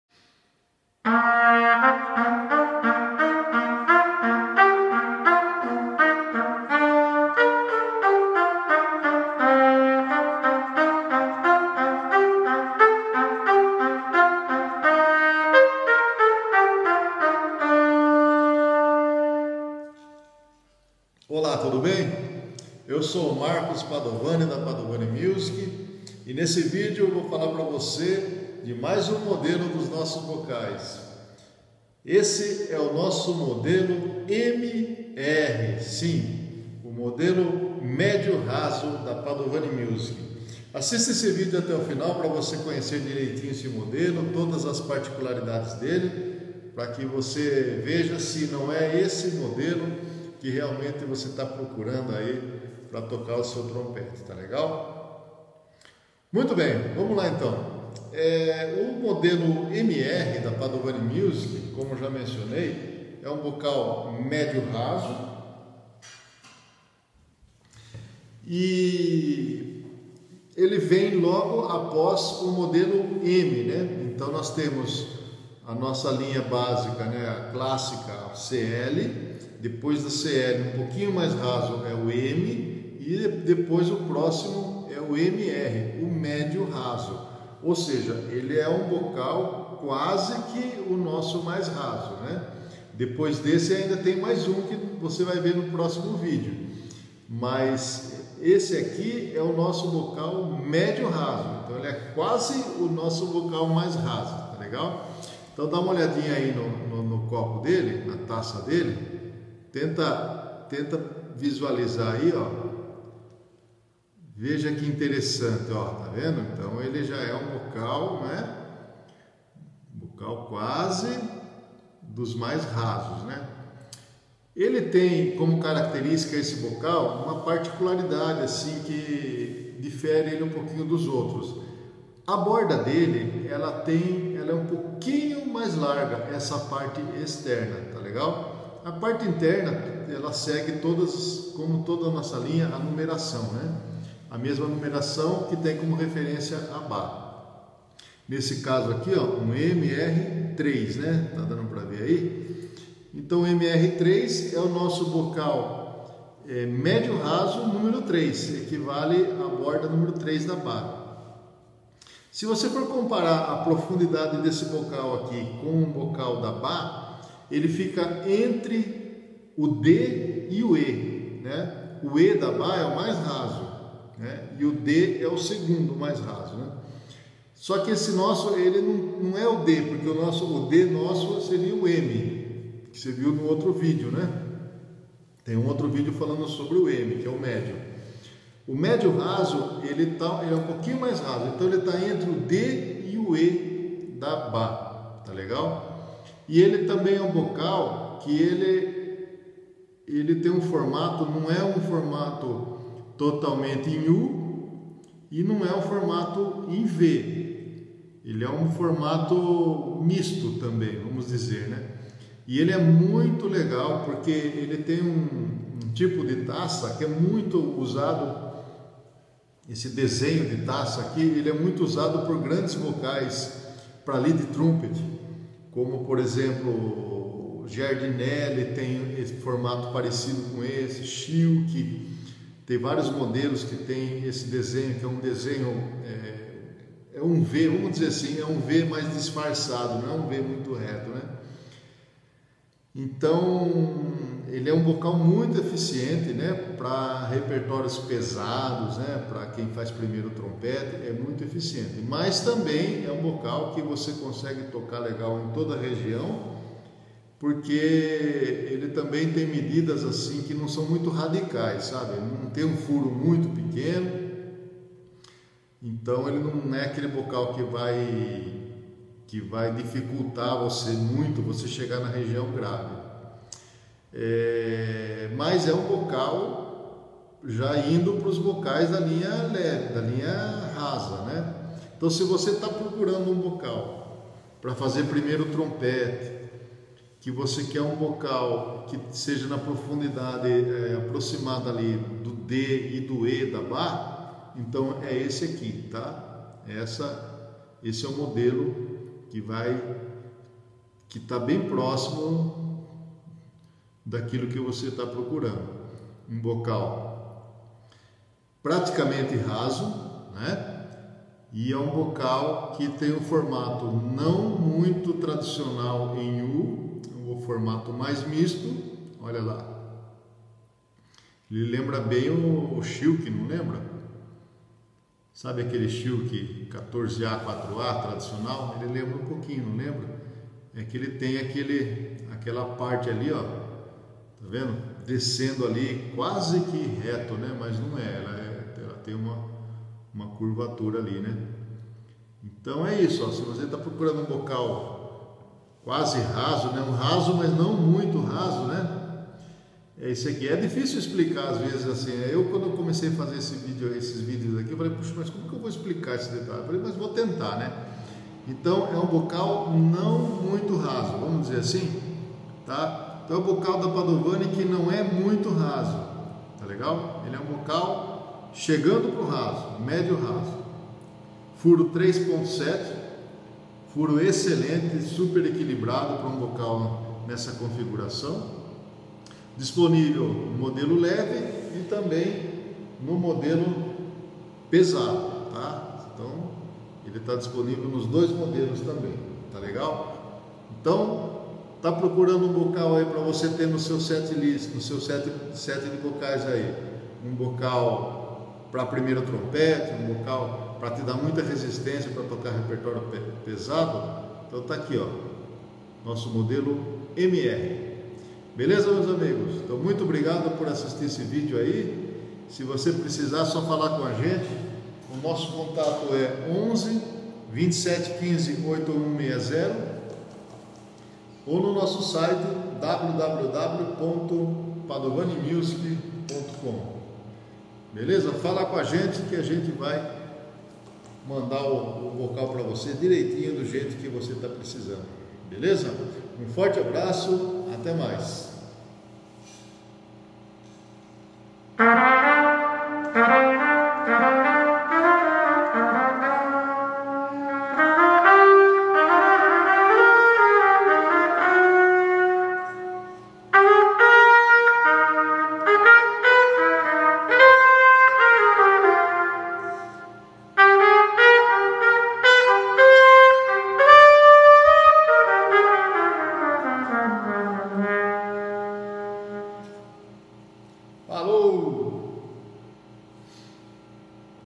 Bocal De Trompete Padovani Linha MR | Trumpet Mouthpiece | Padovani Music
Explicação das características da nossa linha de bocais para trompete " MR "
Profundidade: Médio Raso, entre as linhas "D" e "E".
Se você gostou do conteúdo: Curta 👍 | Comente 👨‍💻 | Compartilhe 🔊

Ative as notificações para receber em primeira mão os próximos conteúdos 🔔🔔🔔🔔 🔔